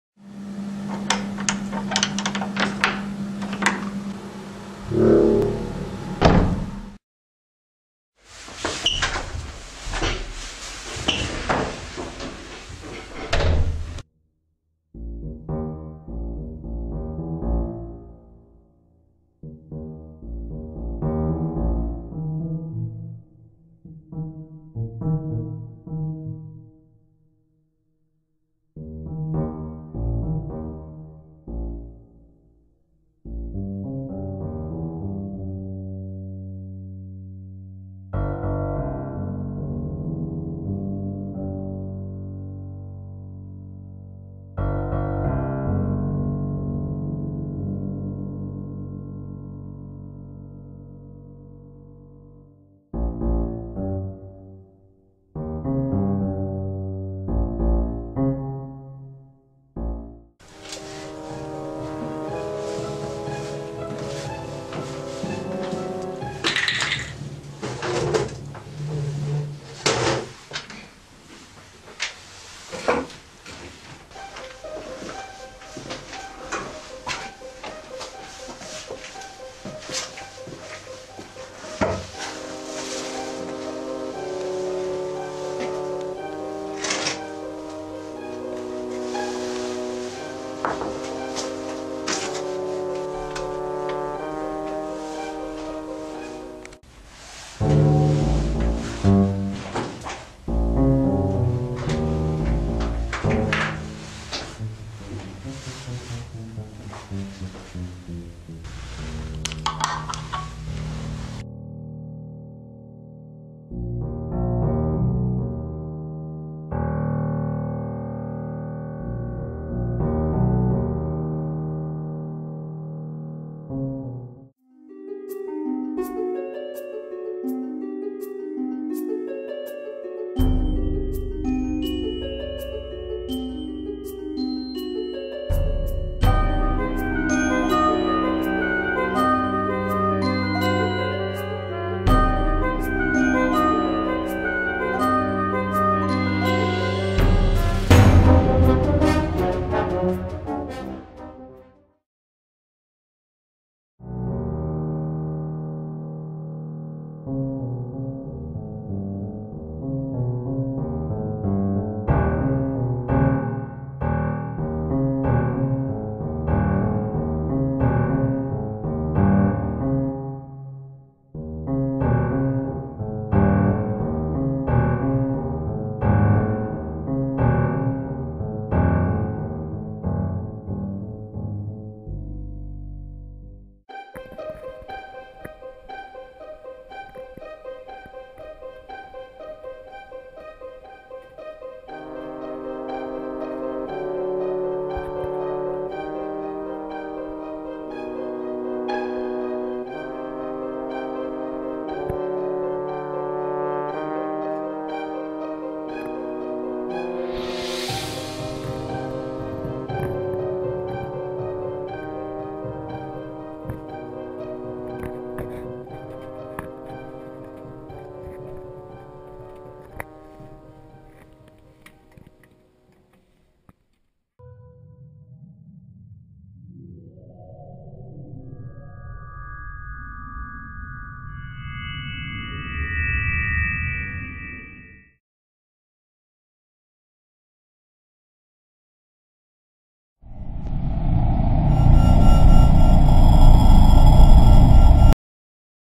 The Strange
Whoever made this will forever remain a mystery...

Re-upload! We missed a few effects and such. Also the rendered file got a little weird and we changed some sounds.

This is a short movie that is kind of strange, we wanted to use a completely empty room in a basement, so we did, but there was not much more we could use around that room. In the corridor outside there were things everywhere, so Christoffer came with an idea, what if that door leads to multiple different rooms? That later became this short film, The Strange.